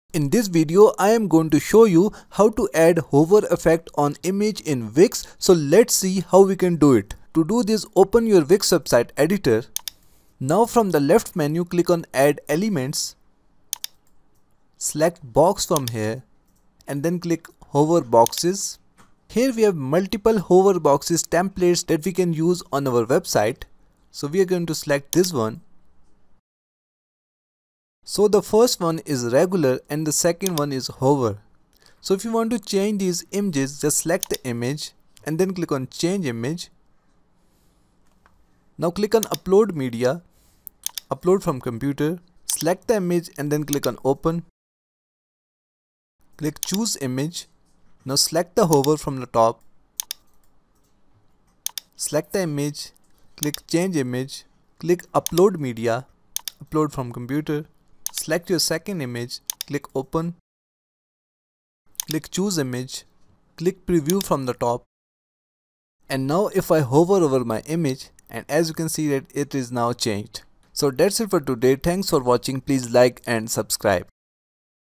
Wix Image Hover Effect Tutorial 2025 | How To Add Hover Effect On Image In Wix | Wix Zoom Image
In this Wix Image Hover Effect tutorial, learn How to add hover effect on image in Wix. By following this video you can easily add zoom image on hover effect in Wix. wix hover effect on image can be very useful if you have a lot of images on your website.

In This Video Tutorial You’ll Learn:
1. How To Add Hover Effect On Image In Wix
2. Wix Image Hover Effect Tutorial

Follow the simple steps to Add Hover Effect On Image In Wix:

Step # 1: Log Into Wix Website Editor
Go to wix.com and log in to your Wix account, now from my sites select your website and edit, now click edit site and now you are in the Wix website editor.

Step # 2: Add Hover Effect On Image In Wix
From the Wix website editor click on add elements select interactively, select hover boxes, choose the hover box and drag it to your page, now choose the before image and hover image and then click on save.

That’s it you have successfully Added a Hover Effect On Image In Wix.